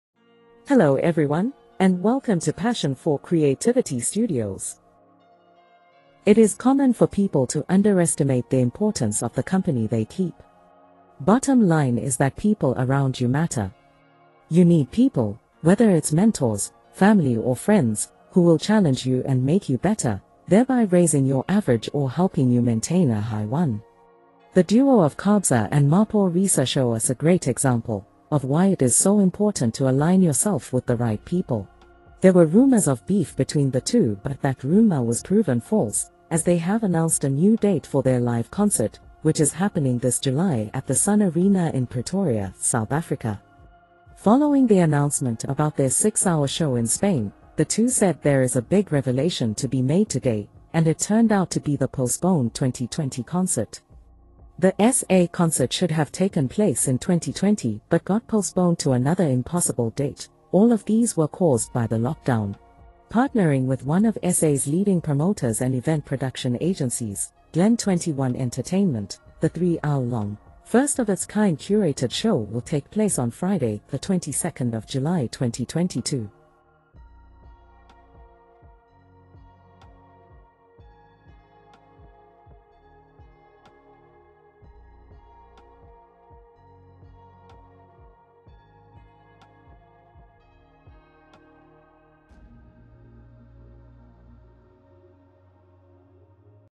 DJ Maphorisa & Kabza De Small Crush Beef rumors
DJ Maphorisa & Kabza De Small Crush beef rumors by setting new date for their Live concert